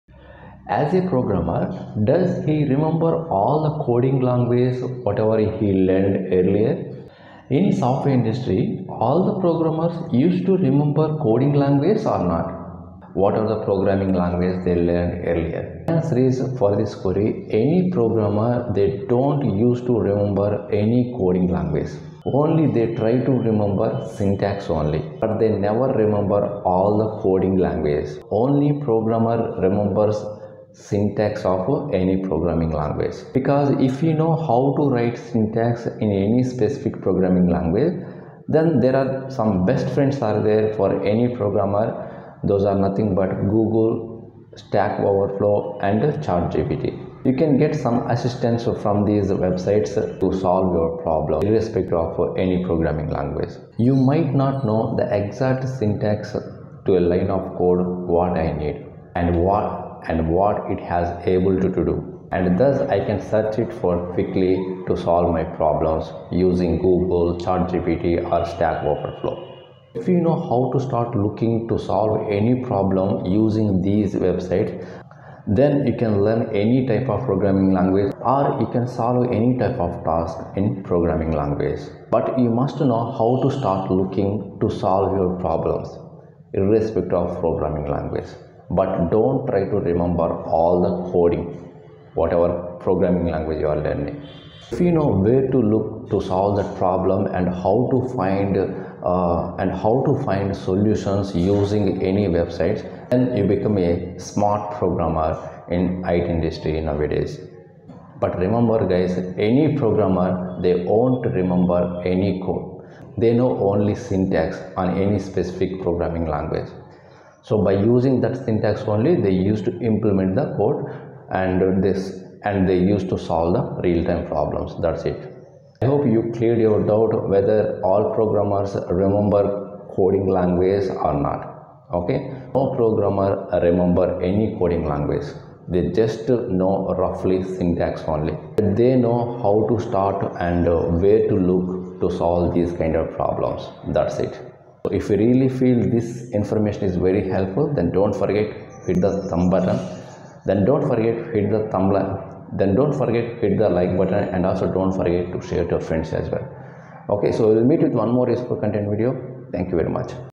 Do All Programmers Remember Coding Languages
Many beginners have doubt who are working in Software Industry do the remember coding languages whatever they learned earlier.